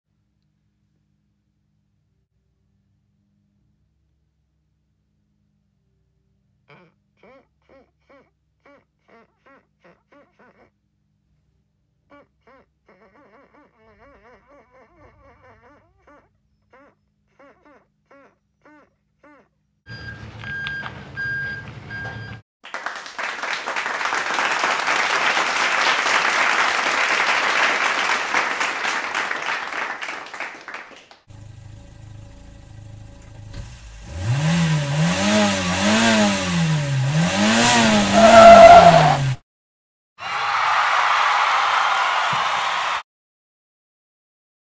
Cars 99, 10, 24 and 37.
Tim Vine adds 4 more cars to his small cars collection. For your information he already had a number 10 but replaced the previous number 10 with this number 10.